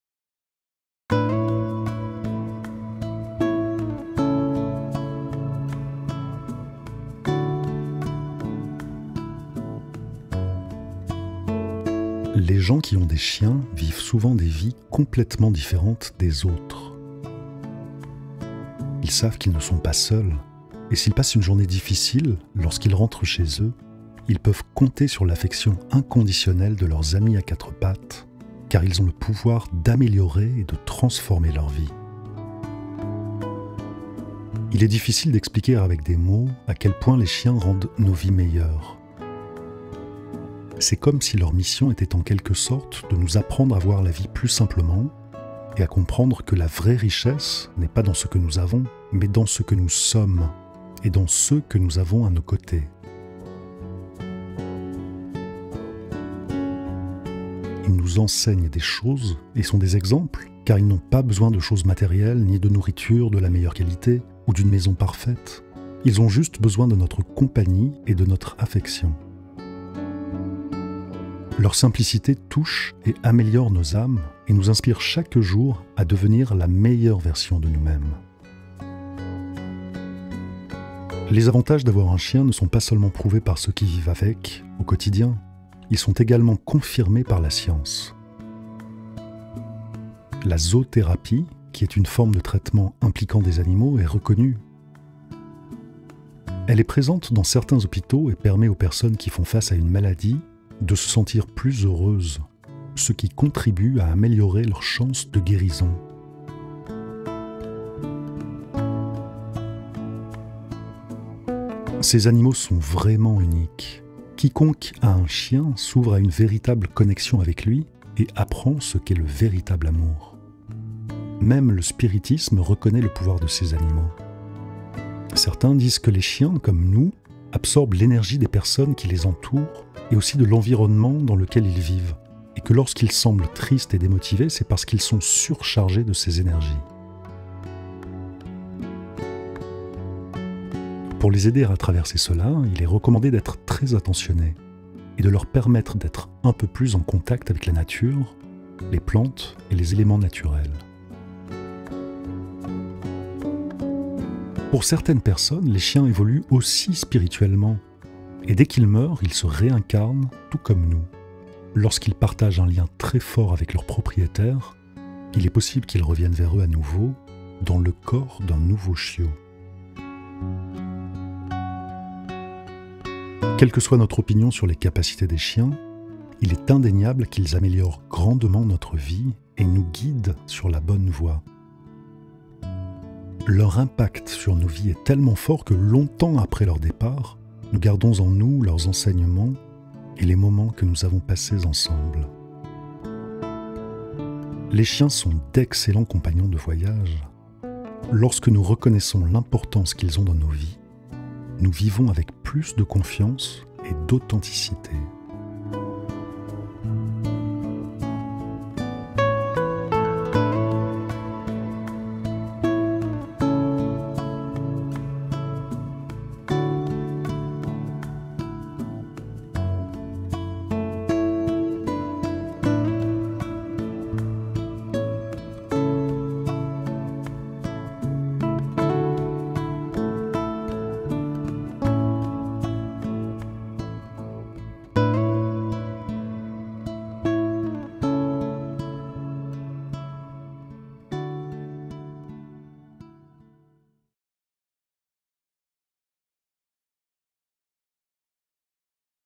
Nos chiens ont une mission spirituelle particulière dans nos vies
Les gens qui ont des chiens vivent souvent des vies complètement différentes des autres.